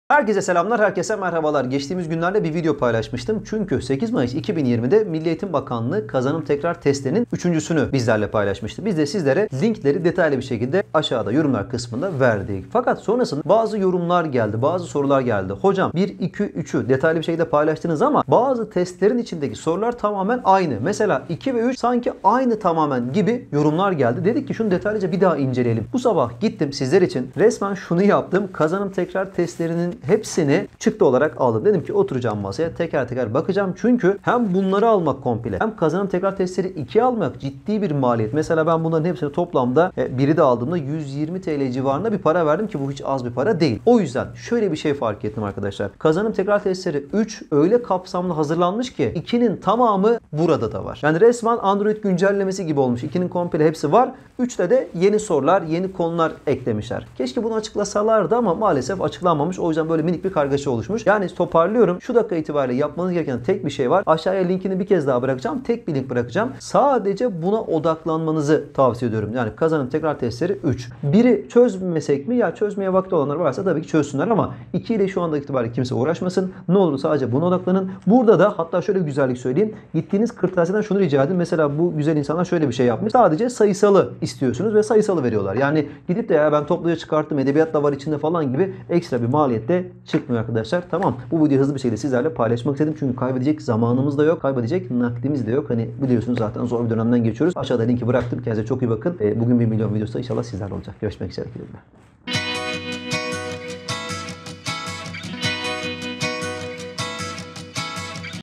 YKS MEB Kazanım Tekrar Testleri 2 ve 3 Aynı mı?
Herkese selamlar herkese merhabalar MEB kazanım testleri için çektiğimiz bir önceki videoda bazı soru işaretleri oluşmuştu. Onları gidermek adına bu videoyu sizinle paylaşıyoruz.